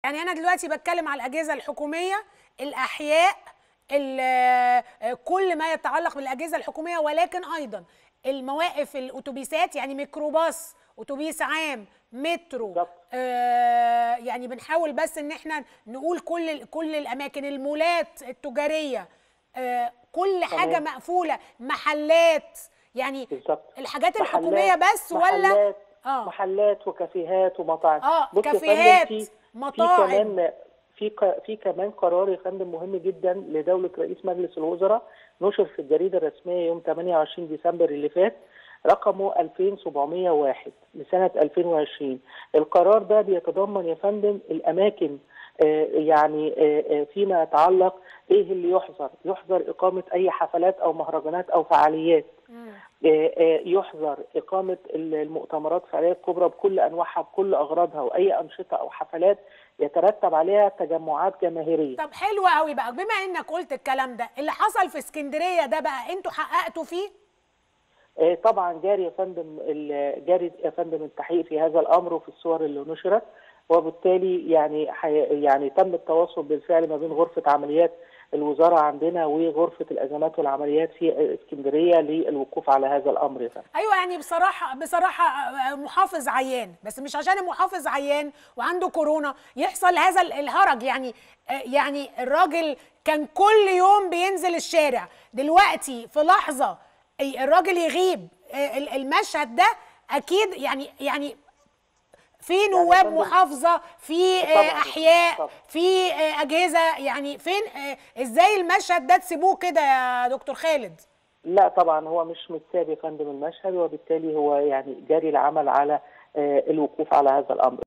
قرار عاجل عن تطبيق غرامة عدم إرتداء الكمامة وتعليق التنمية المحلية على تحقيقات احتفالات كوبرى ستانلي
جزء من حلقة اليوم -  قرار عاجل عن تطبيق غرامة عدم إرتداء الكمامة وتعليق التنمية المحلية على تحقيقات احتفالات كوبرى ستانلي